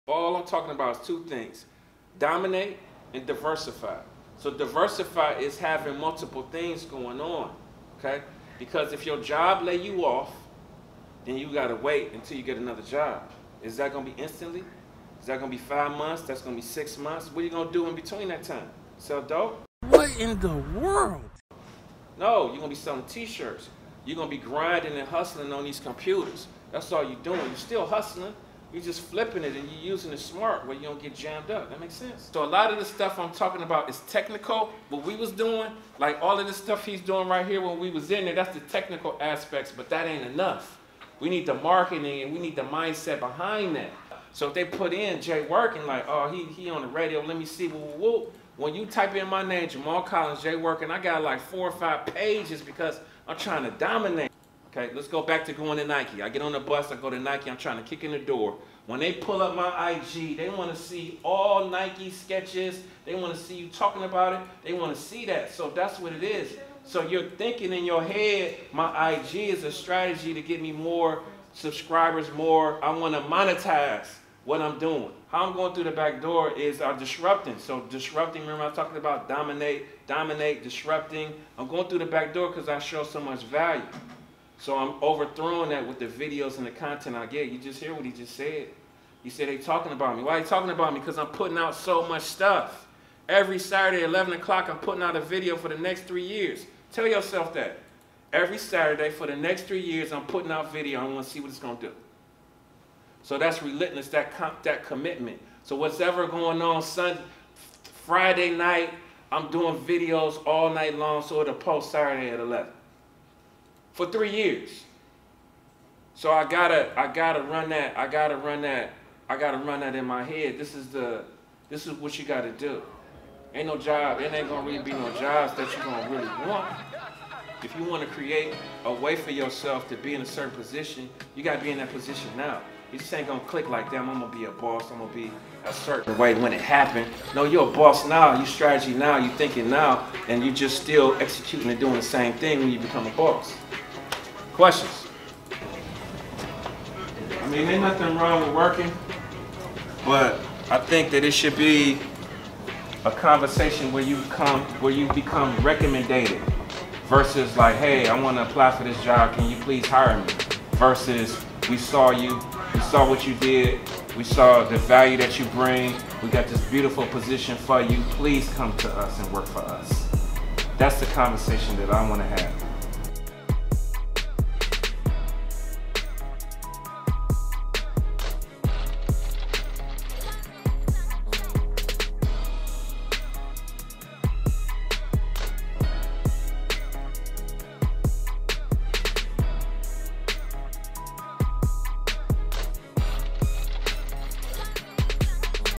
Diversify & Dominate
The day and the life of jayworking ride with me to get an inside look at the After School Graphic Design Program I started in Cleveland Ohio. I go to two different locations a day.  I teach at-risk youth I show them how to have the right attitude so that they can get into a position of power and how to move as an young entrepreneur. My goal is to not only make good graphic designers but good creative thinkers.

New Website